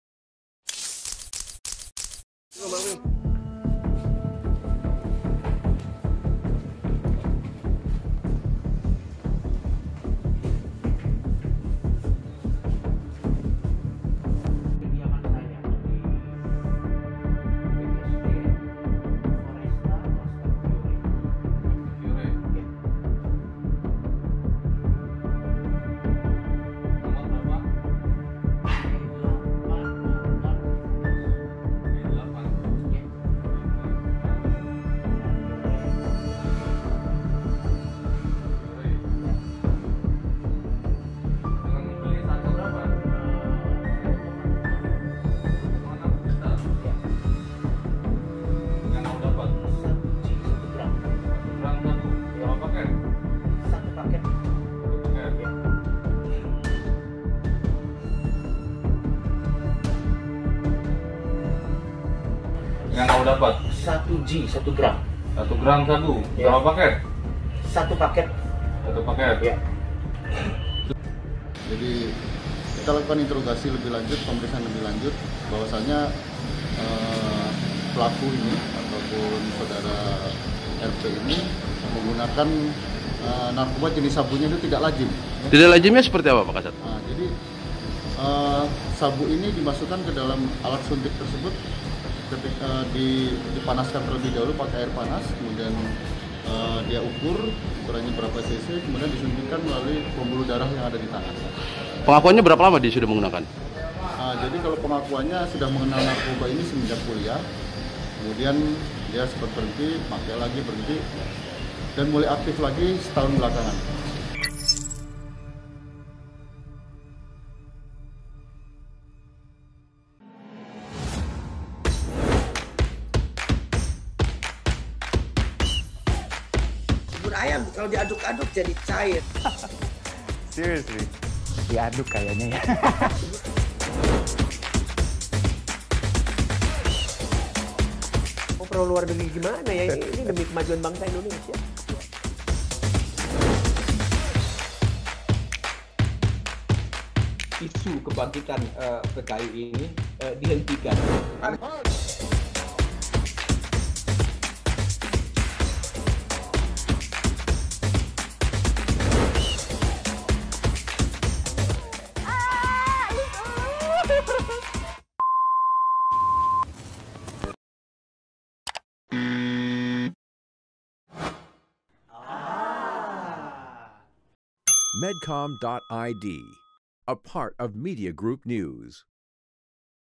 Coki Pardede Suntikan Sabu ke Pembuluh Darah dan Dubur
Komika Coki Pardede masih menjalani pemeriksaan di Polres Metro Tangerang Kota usai ditangkap karena penyalahgunaan narkoba jenis sabu suntik. Coki mengaku mendapat sabu dari teman wanitanya yang ikut diamankan.

Coki sendiri mengaku menggunakan narkoba jenis sabu yang dibeli dengan harga Rp1,2 juta dari teman wanitanya yang turut diamankan petugas. Kasat Narkoba Polres Metro Tangerang Kota AKBP Pratomo Widodo mengatakan akan menaikkan status coki pardede hari ini.

Berdasarkan pengakuan Coki, Pratomo menjelaskan, yang bersangkutan menggunakan suntikan melalui proses pencairan sabu terlebih dahulu. Setelah dilarutkan, hasil proses tersebut dimasukan ke pembuluh darah dengan menggunakan jarum suntik.

Sedangkan, metode melalui dubur dilakukan jika Coki merasa belum mencapai kenikmatan. Coki telah aktif mengonsumsi sabu sejak masih berkuliah. Walau sempat berhenti,  ia kembali menggunakan haram tersebut belakangan ini. Nanang Yudistira/Metro TV.

Hai Sobat Medcom, terima kasih sudah menjadikan Medcom.id sebagai referensi terbaikmu.

Kami ingin lebih mengenali kebutuhanmu. Bantu kami mengisi angket ini yuk